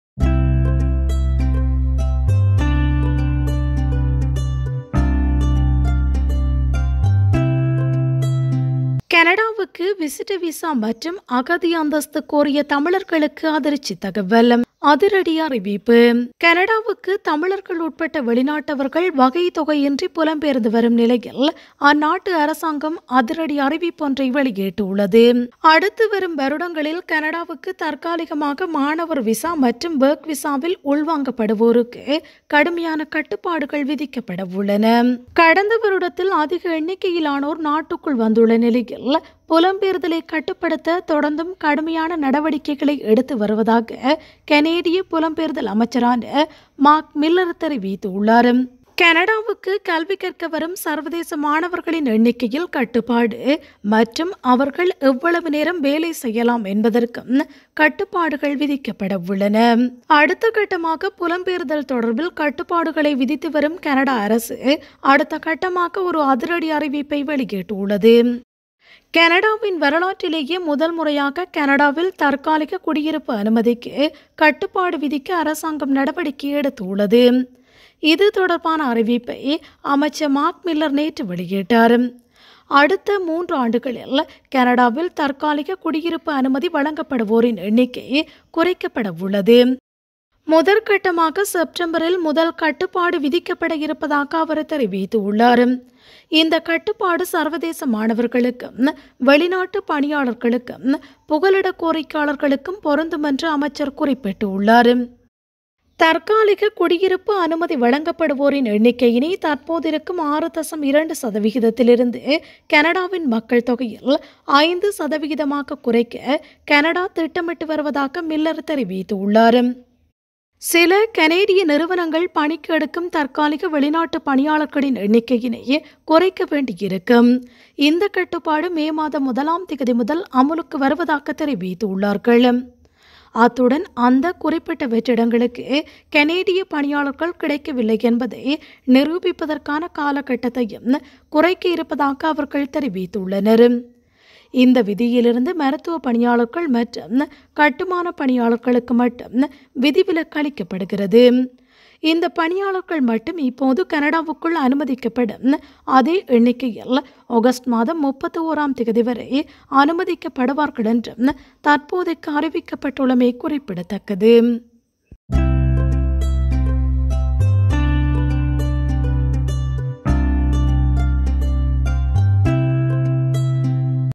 கனடாவுக்கு விசிட்டர் விசா, அகதி அந்தஸ்து கோரிய தமிழர்களுக்கு அதிர்ச்சித் தகவல் - அதிரடி அறிவிப்பு
கனடாவுக்கு தமிழர்கள் உட்பட வெளிநாட்டவர்கள் வகைதொகையின்றி புலம்பெயர்ந்து வரும் நிலையில், அந்நாட்டு அரசாங்கம் அதிரடி அறிவிப்பு ஒன்றை வெளியிட்டுள்ளது.